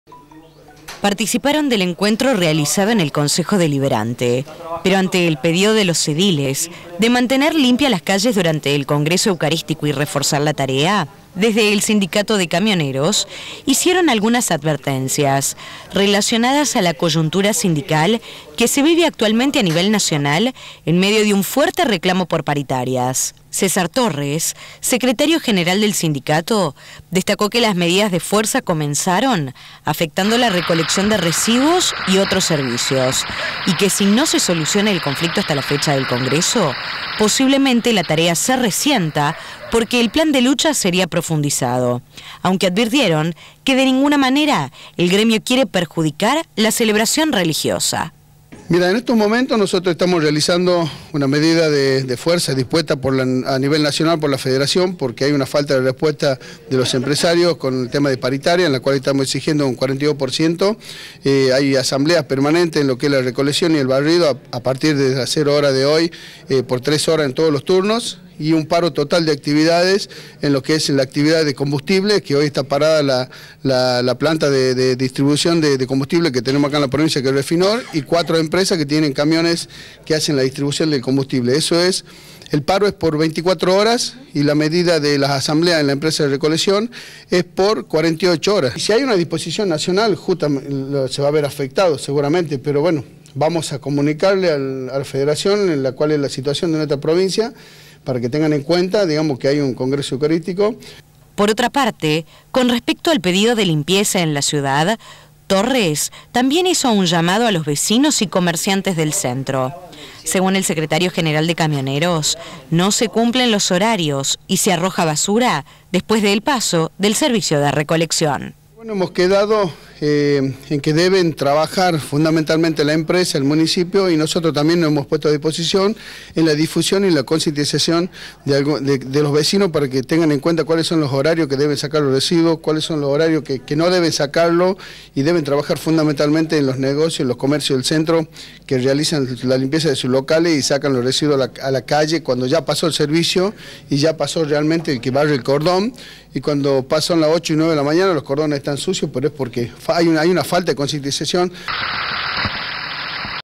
Paro de Camioneros
La modalidad de protesta afecta el servicio de recolección, barrido, y hasta combustibles; es que el sindicato de camioneros, en reclamo por paritarias y un aumento del 42% comenzó un plan de lucha de 48 horas en principio.
Esto podría profundizarse y extenderse de ser así, hablaran con la Federación para que en Tucumán el evento religioso sea tenido en cuenta analizaron el pedido formulado por los ediles.